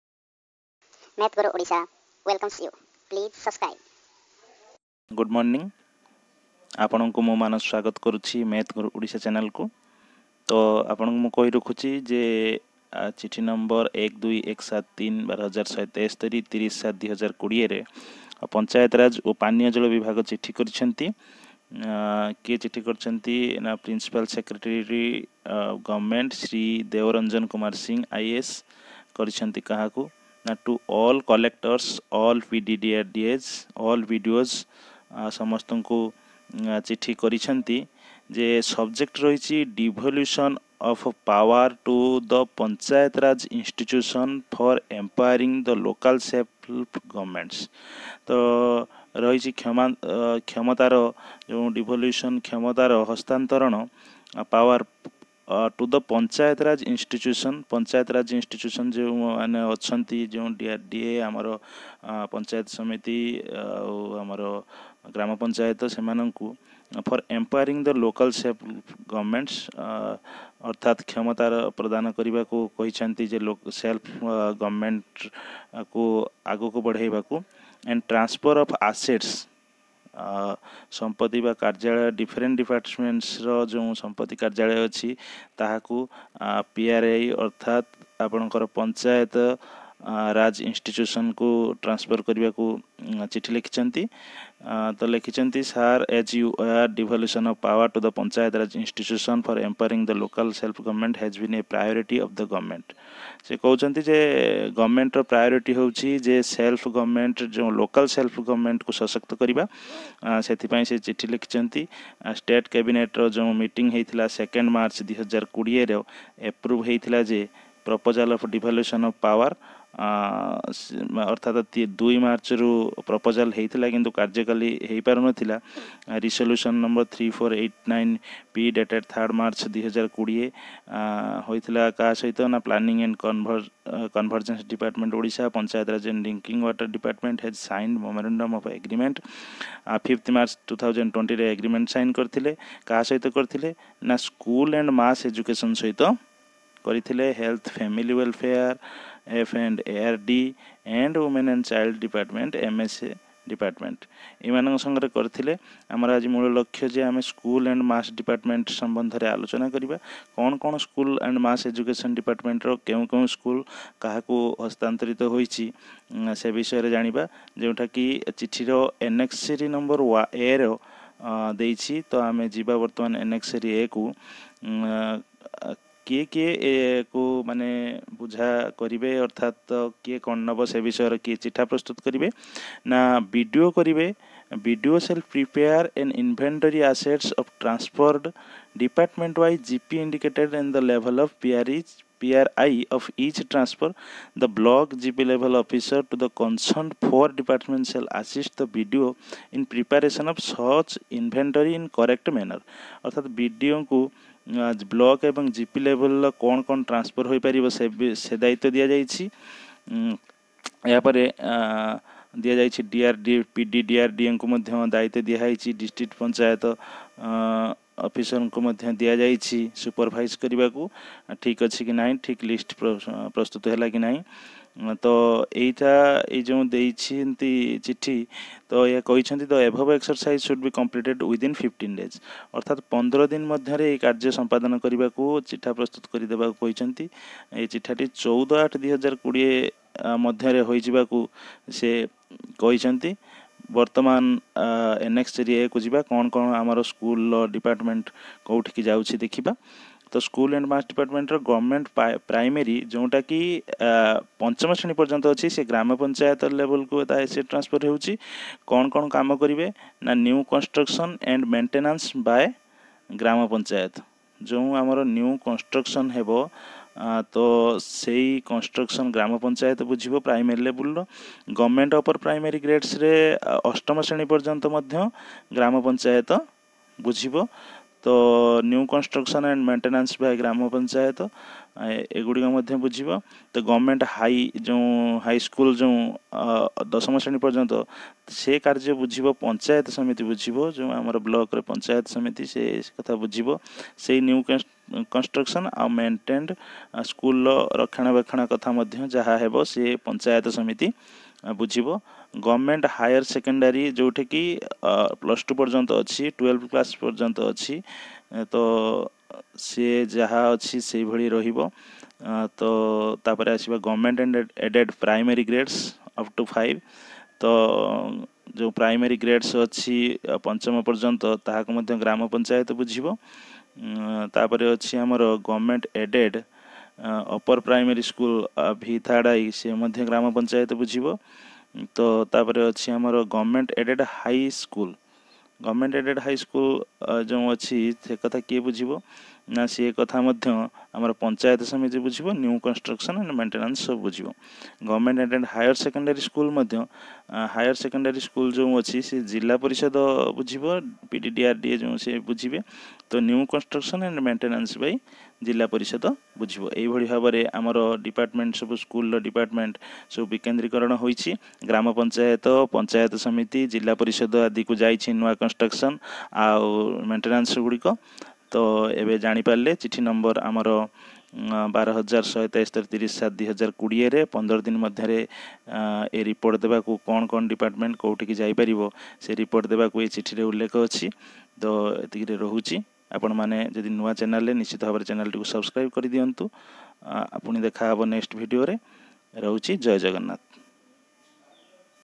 BREAKING NEWS. ସ୍କୁଲ ଚାବି ଏବେ ପଂଚାୟତ ରେ .. DEVOLUTION OF POWER TO THE PRI FOR EMPOWERING LOCAL GOVT
OUR OTHER POPULAR VIDEO. - ଘନ ମୂଳ ନିର୍ଣ୍ଣୟ ମାତ୍ର 4 ସେକେଣ୍ଡ ରେ  -

SERVE INDIA
LOVE INDIA.